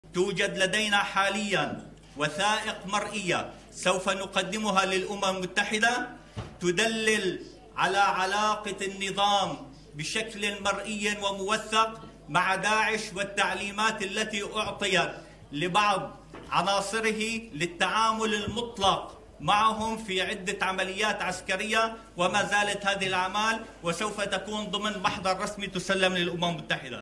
المعارضة السورية ستقدم "وثائق مرئية" عن علاقة النظام بداعش
شاهد آيضا:
قيادي بعثي يفضح تاريخ آل الأسد!؟
قال عضو وفد المعارضة السورية في مباحثات جنيف، العقيد فاتح حسون، السبت، إن لدى المعارضة السورية أدلة "مرئية" تؤكد وجود علاقة بين "النظام" بداعش.

وأضاف حسون، في مؤتمر صحفي للمعارضة من جنيف، "توجد لدينا حاليا وثائق مرئية، سوف نقدمها للأمم المتحدة تدلل على علاقة النظام مع داعش".

ومضى يقول إن "هناك تعليمات أعطيت لبعض عناصر النظام للتعامل المطلق مع التنظيم المتطرف في عدة عمليات عسكرية. ولا تزال هذا الأعمال موجودة".

واتهم حسون، وهو ضابط سابق منشق عن النظام، بتدبير تفجيرات حمص، التي أدت إلى مقتل مسؤولين أمنيين كبيرين، السبت.

وقال إن "ما جرى تصفية من قبل النظام للمطلوبين لمحاكم دولية، وعلى رأسهم اللواء الذي تم قتله، وهو متهم في قضية مقتل رئيس الوزراء اللبناني رفيق الحريري".

واستهدفت تفجيرات، تبنتها "جبهة النصرة"، مقري أمن الدولة والأمن العسكري، وأسفرت عن مقتل رئيس فرع الأمن العسكري التابع للنظام حسن دعبول، بالإضافة إلى آخرين.

وأوضح حسون أن المنطقة التي يوجد بها الفرع الأمني "منطقة أمنية شديدة الحراسة، وخاضعة للمراقبة الدائمة، ولا يمكن أن تتم أي عملية أمنية إلا بتسهيلات من قوى أمنية أخرى تمتلك نفوذا للوصول إلى قلب هذه المناطق".

وأكد أن "هذه المناطق لا يوجد فيها أي وجود عسكري لفصائل مسلحة، وأن أقرب منطقة فيها (قريبة من مكان التفجيرات) تخضع لسيطرة المعارضة هي حي الوعر المحاصر منذ 3 أعوام".